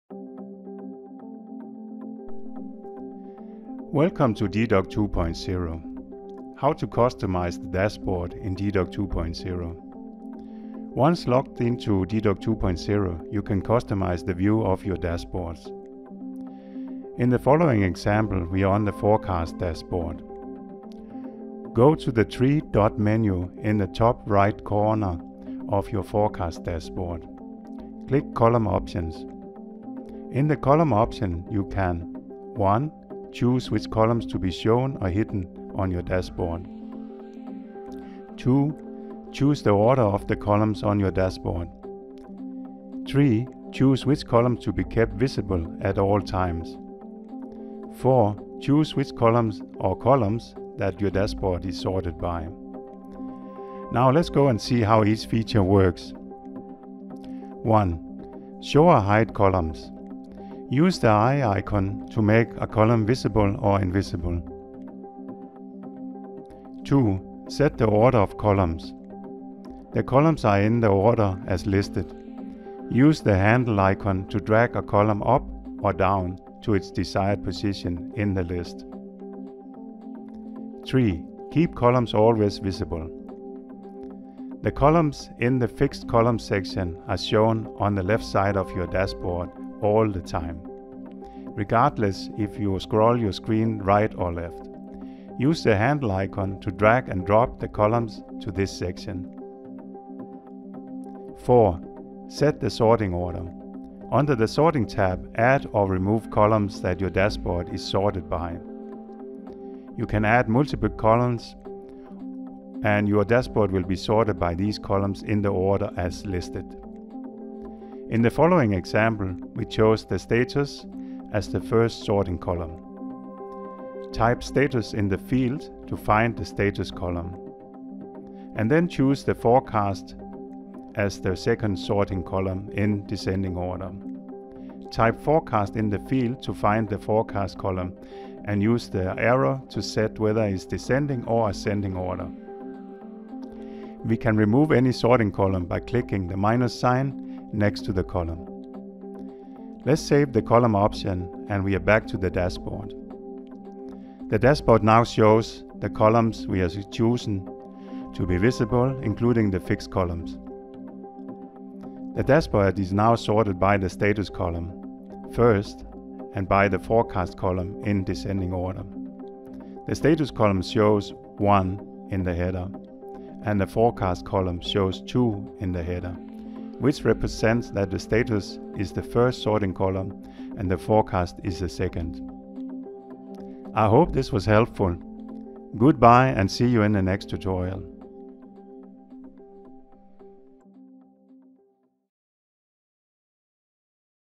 How to customize the dashboard in gDoc 2.0
0:00 - Welcome
0:21 - The three Dot menu
0:29 - Column options
0:57 - How each Colum feature works
1:41 - Sorting order of Columns
1:57 - Example reordering the sorting of Columns
2:34 - Back in the Dashboards
3:05 - Goodbye

The official style to be followed in drafting and editing United Nations documents